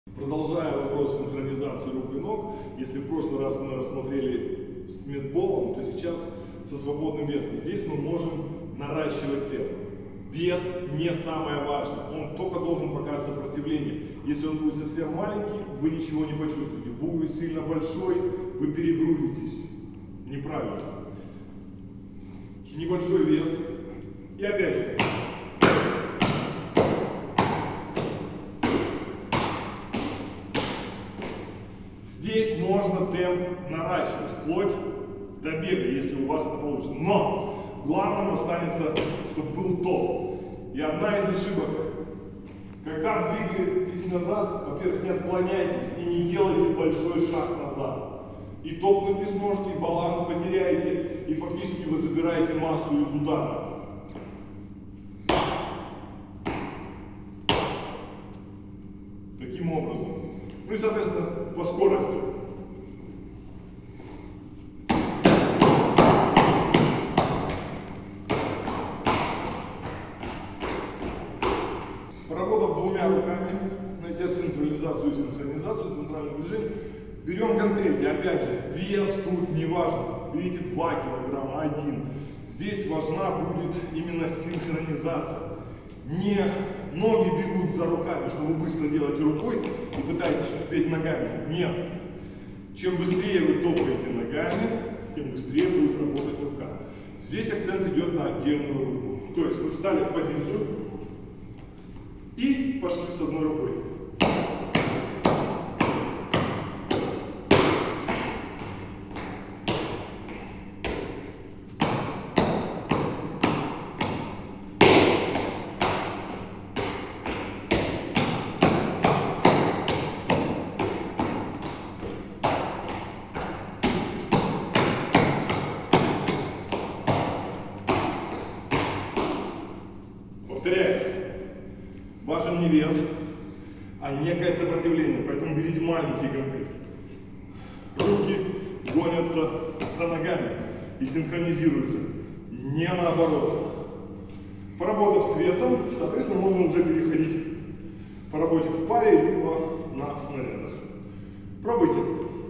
Синхронизация в ударе 2 Гантели
Дополнительное утяжеление способствует быстрому погружению в тело основных принципов синхронизации для повышения качества выпоняемых действий.